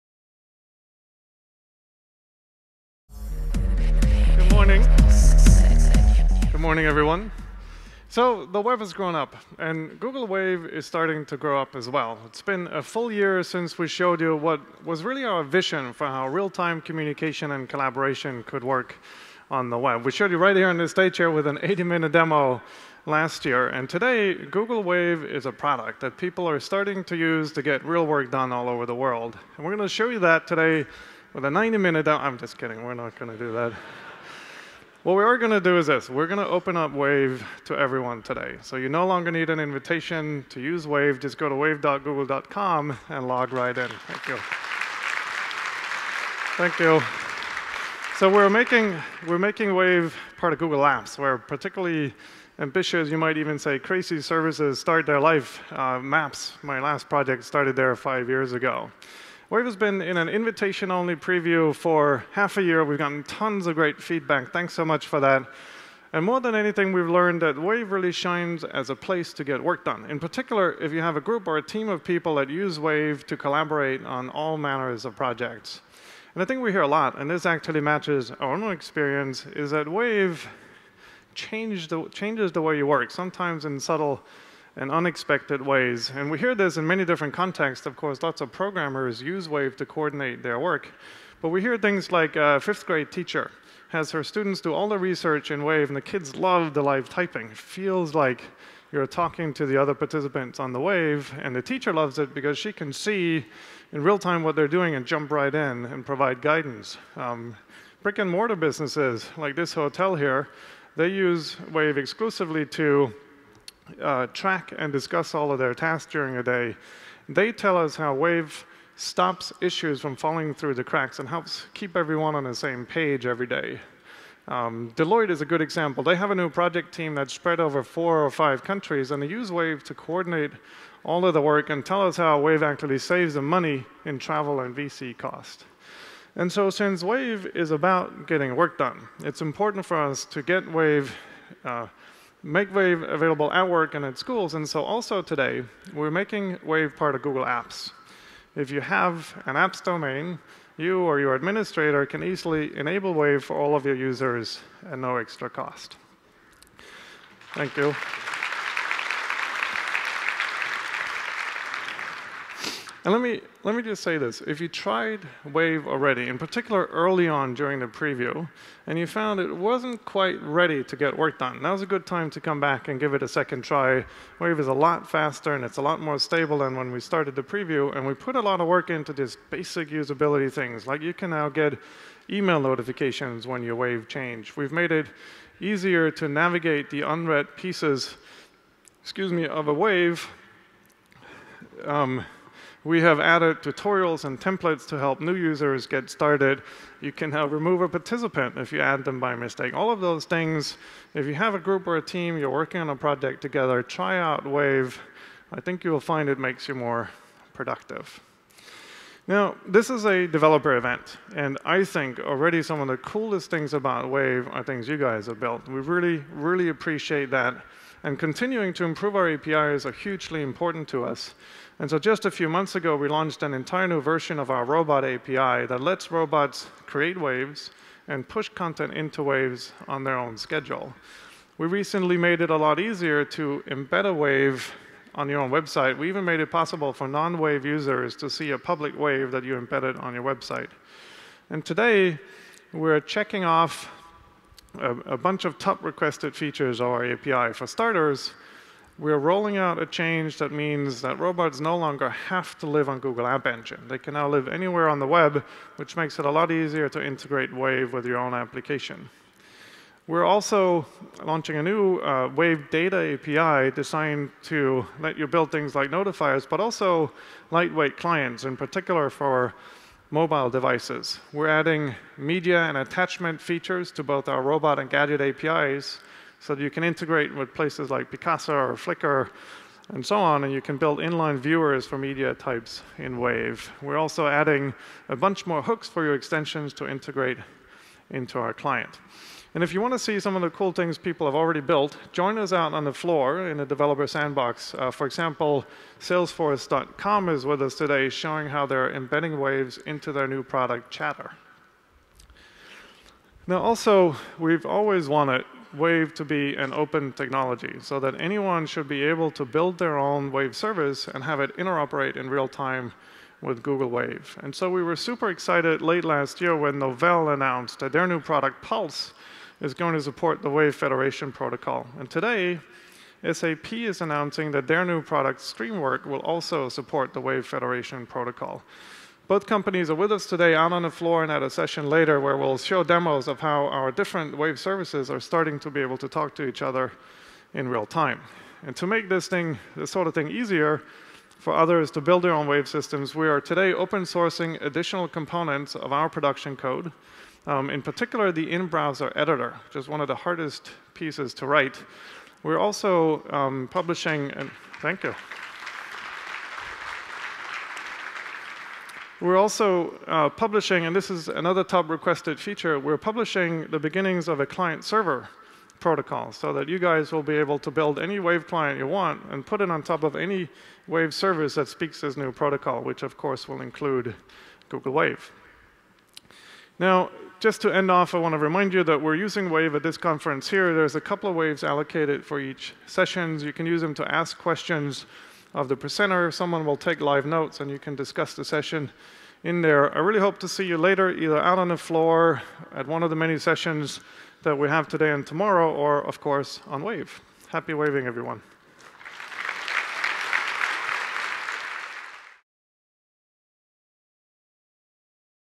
Google Wave at Google IO 2010 Keynote
An update on Google Wave's first year and new announcements by Lars Rasmussen at Google IO 2010.